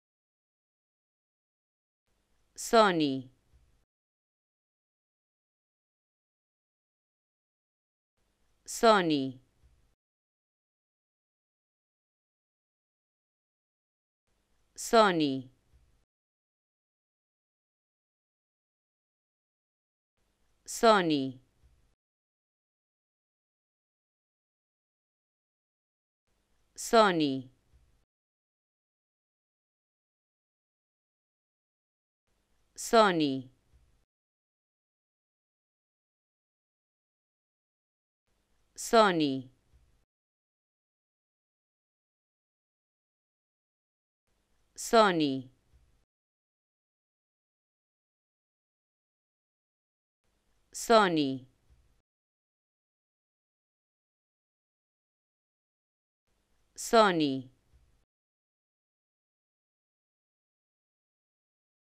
Como Pronunciar ''Sony'' Correctamente en Español
Escucha la pronunciación de ''Sony'' y aprende a decirlo correctamente en Español.